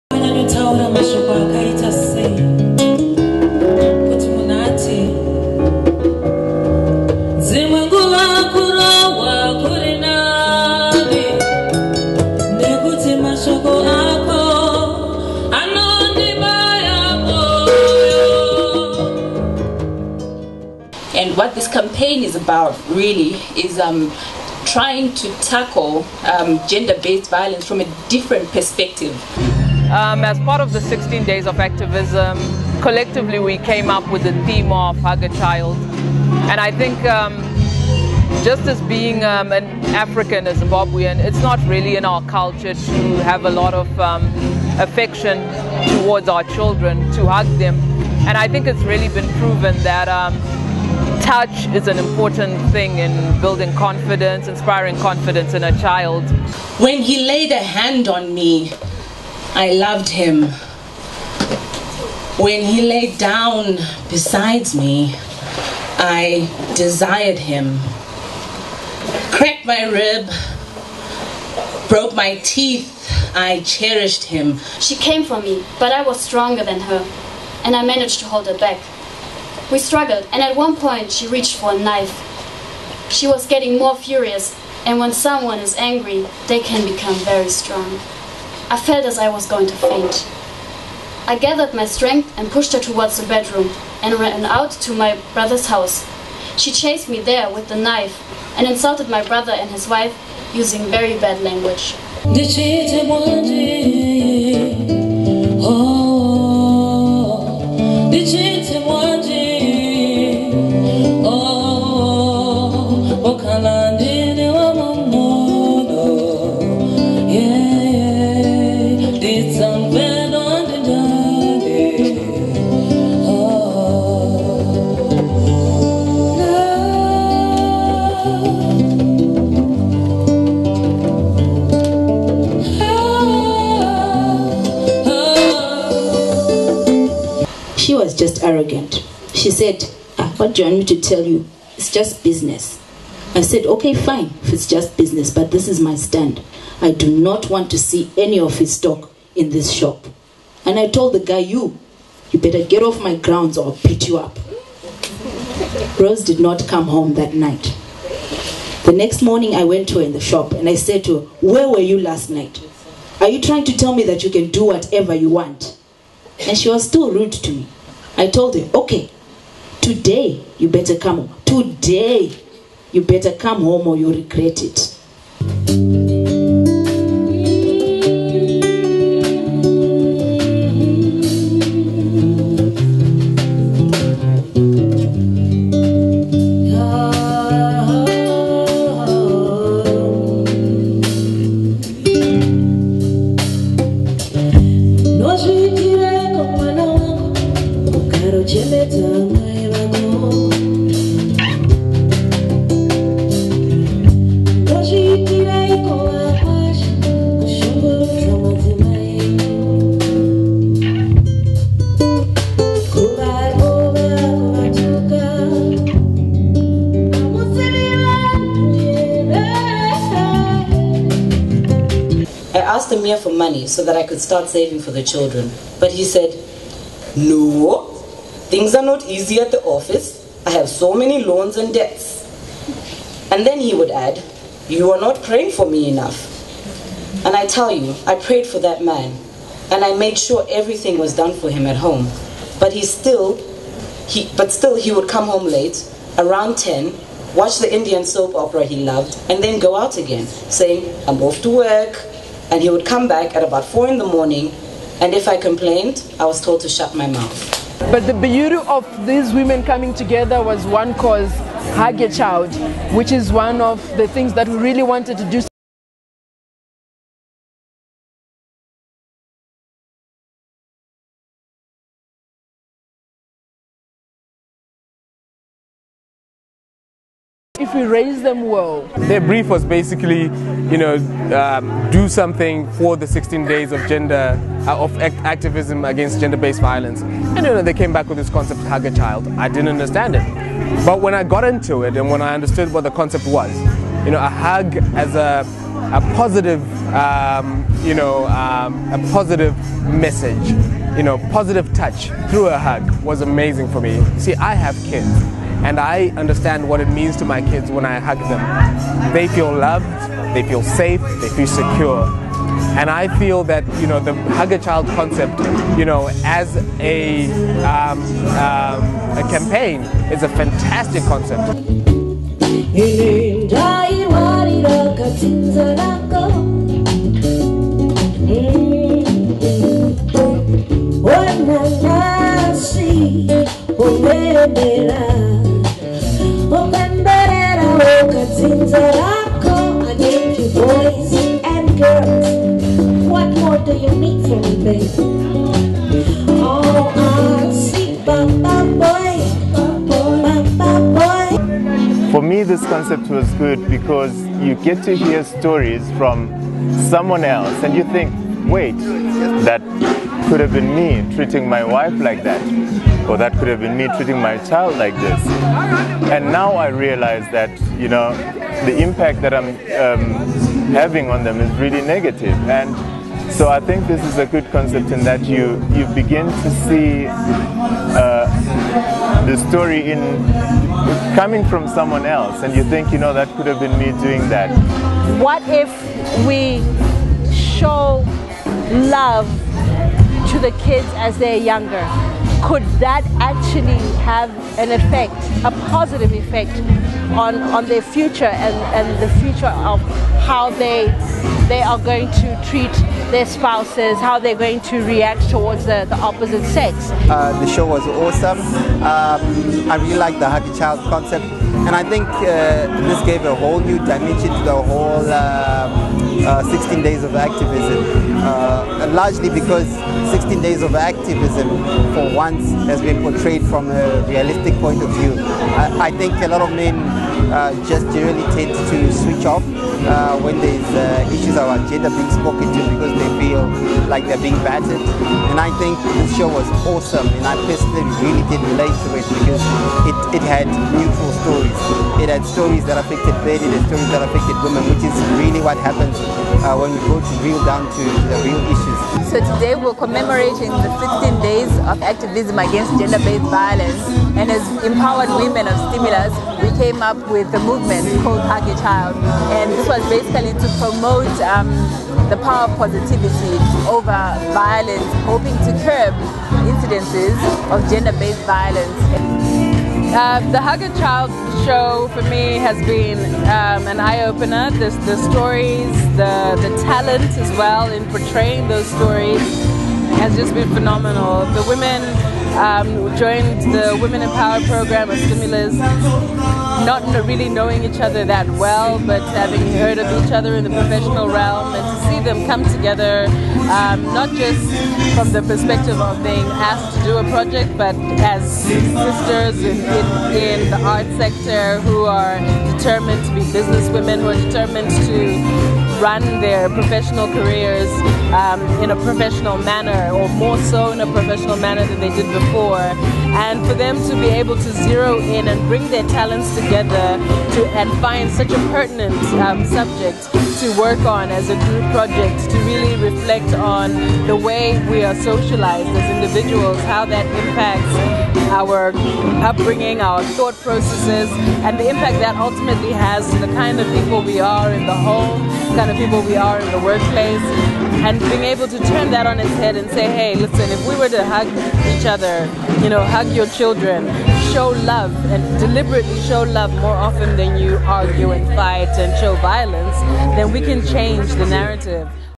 Hug A child
An amazing concept thought up by the Stimulus Women Empowerment Program, here are some highlights of the evening of the concept launch during 16 Days of Activism Against Gender Based Violence